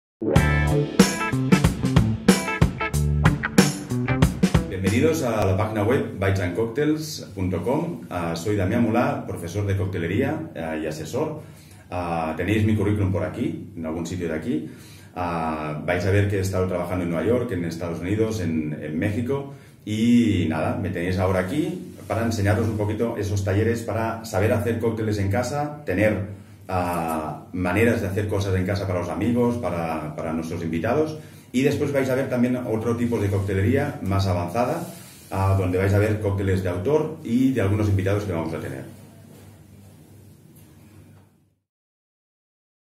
Presentación bitesandcocktails.com
Presentación de la plataforma de talleres de coctelería online bitesandcocktails.com
Con Damià Mulà